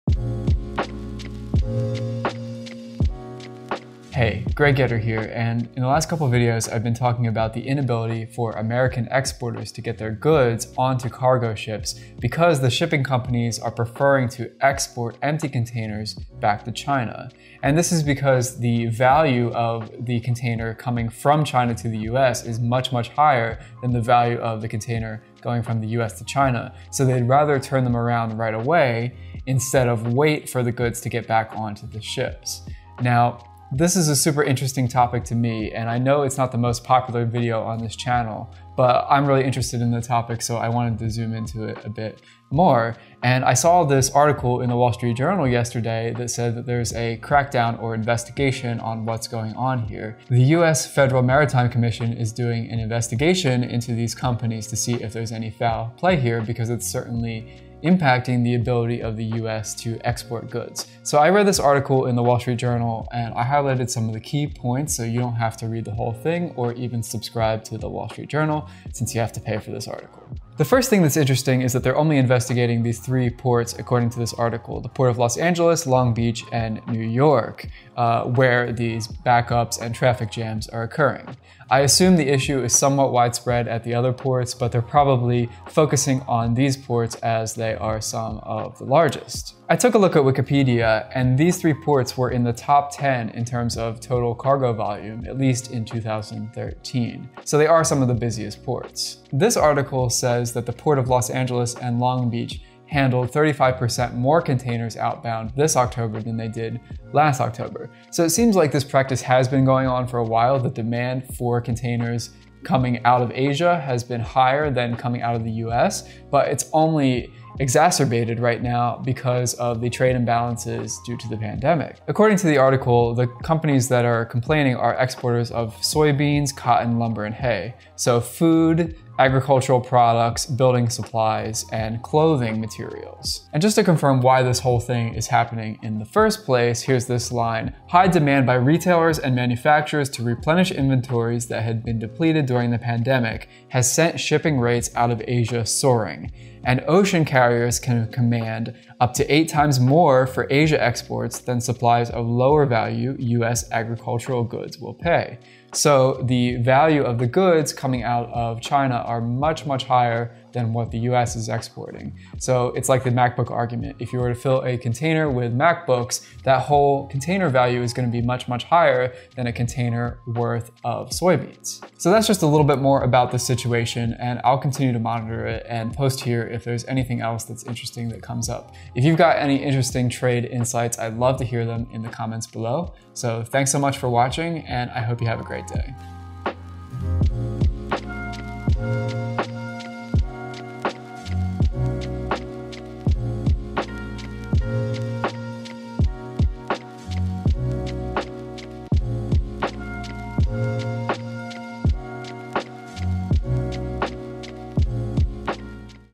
Investigation Launched Into Empty Shipping Containers
The U.S. Federal Maritime Commission is launching an investigation into empty shipping containers.

USEFUL LINKS:
• Read the article in the Wall Street Journal:

Thumbnail photo by Erwan Hesry on Unsplash